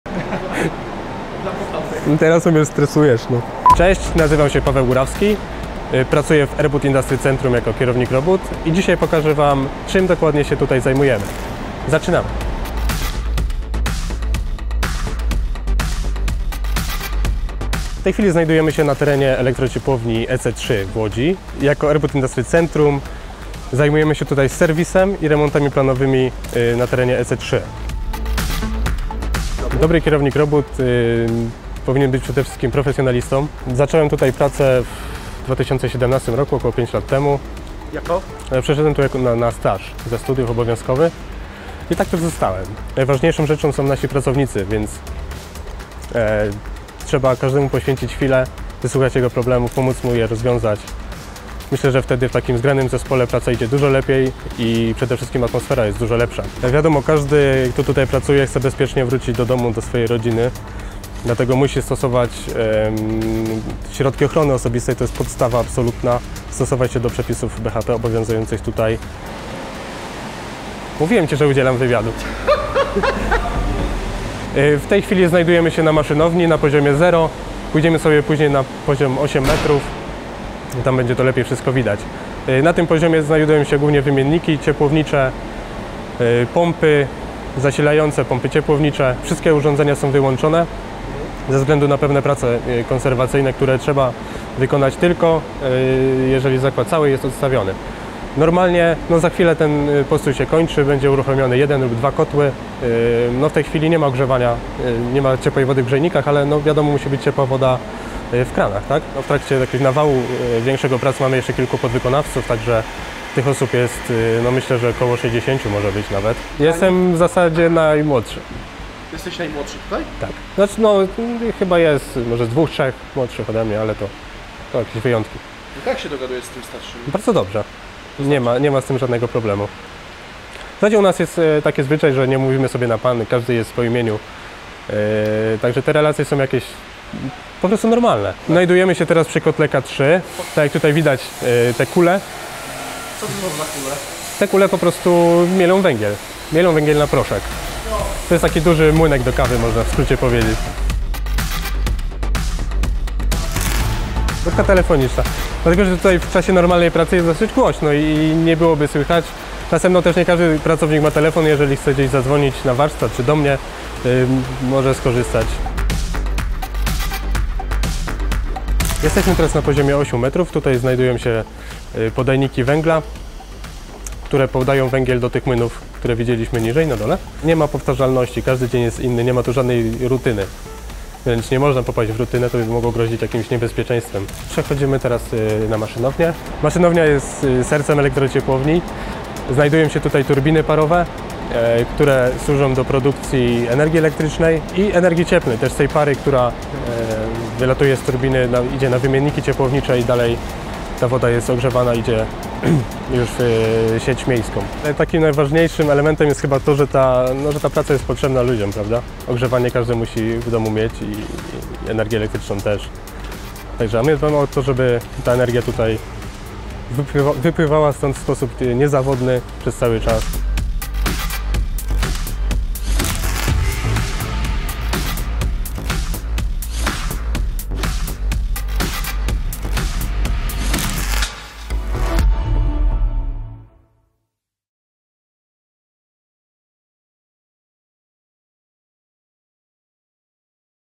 Jakie wykonujemy prace w tym zakładzie? Opowiada Kierownik Robót [PL, DE, ENG]
Odwiedziliśmy Elektrociepłownię EC3 w Łodzi, gdzie jako Erbud Industry Centrum zajmujemy się serwisem i remontami planowymi. Po terenie oprowadził nas kierownik robót Paweł Górawski, jeden z najmłodszych pracowników naszej spółki na terenie zakładu w Łodzi.